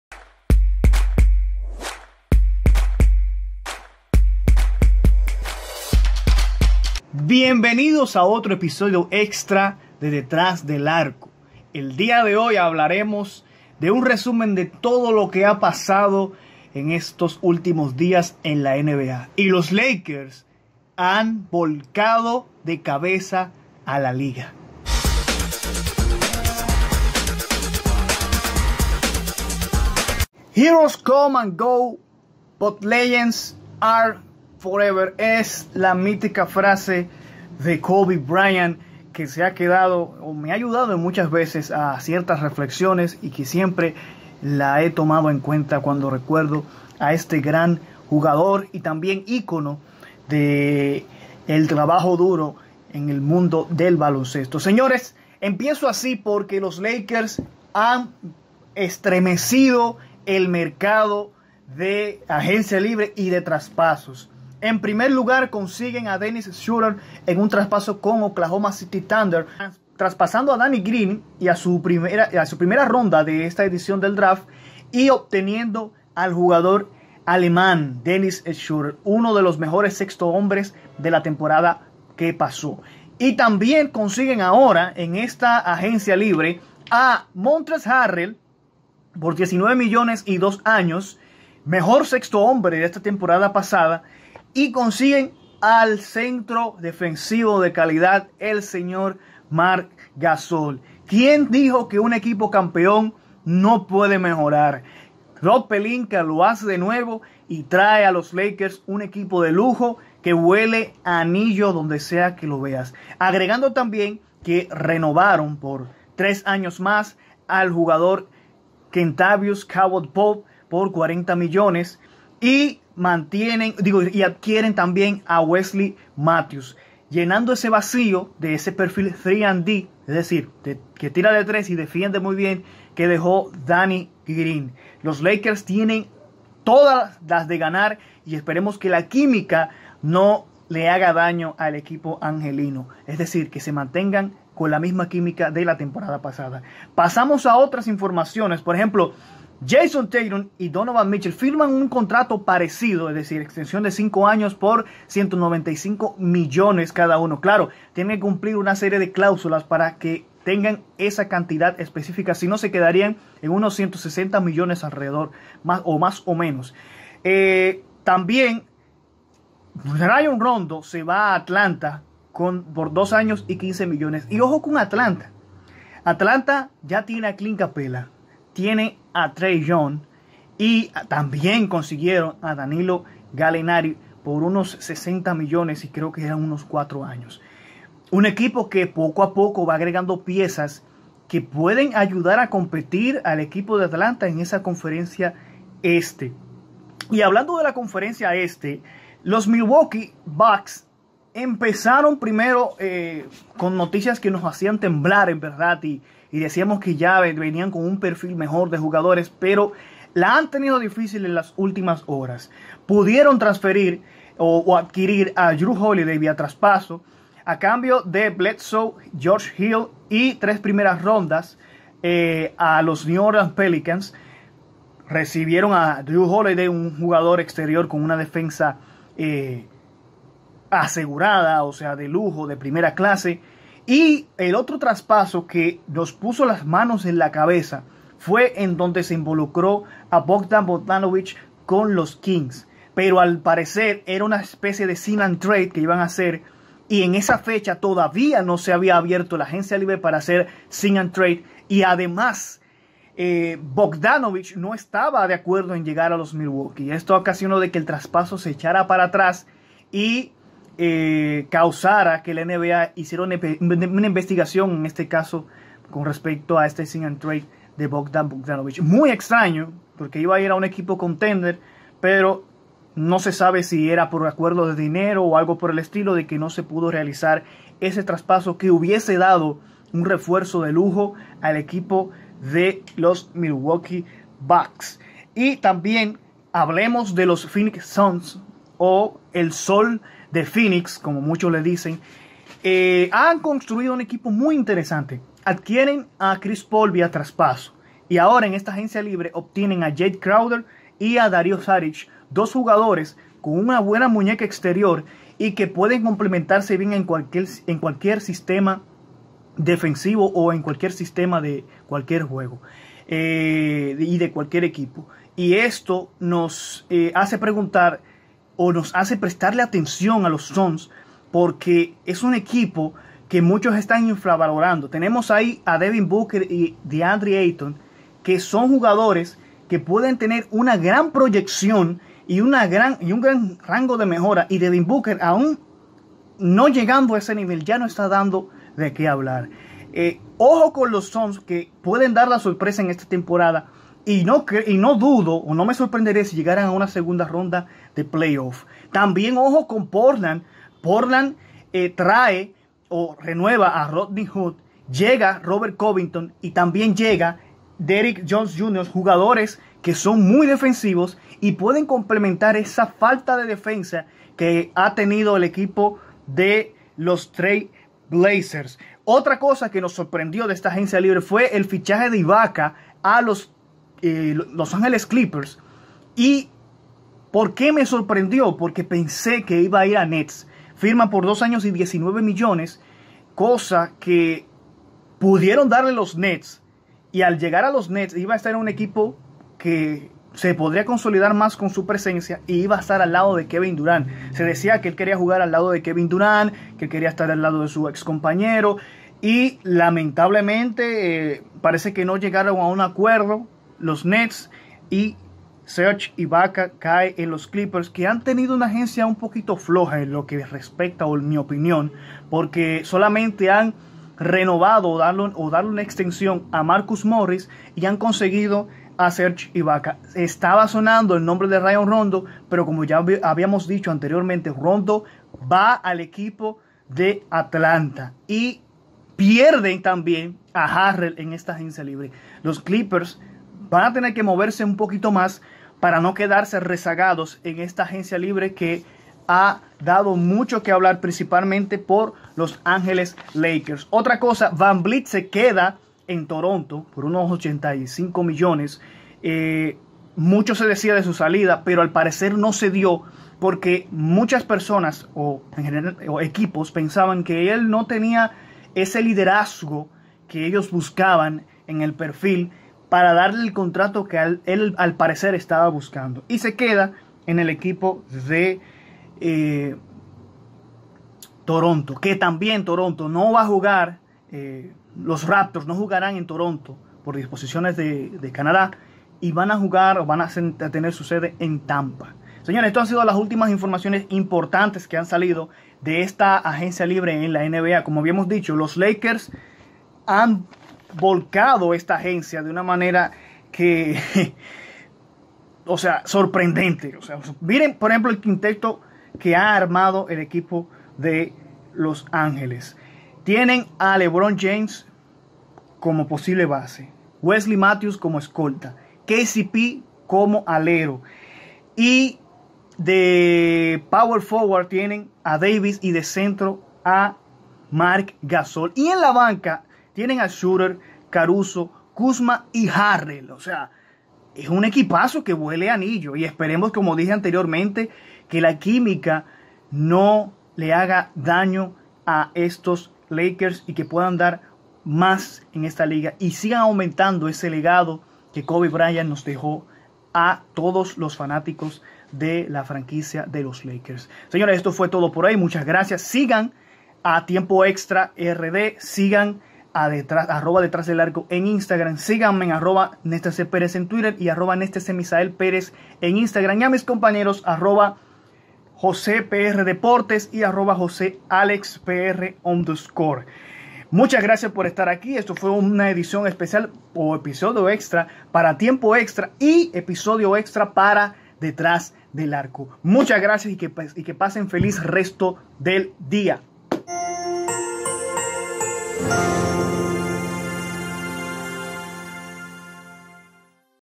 Agencia libre de la NBA liderada por los Lakers ¿Cuál es su posible quinteto? Movimientos destacados
Análisis y repaso de los principales movimientos de la agencia libre de la NBA
Posible quinteto de los Lakers para la temporada

Conducido por:
Nestese Pérez (@NesteseMisaelPerez)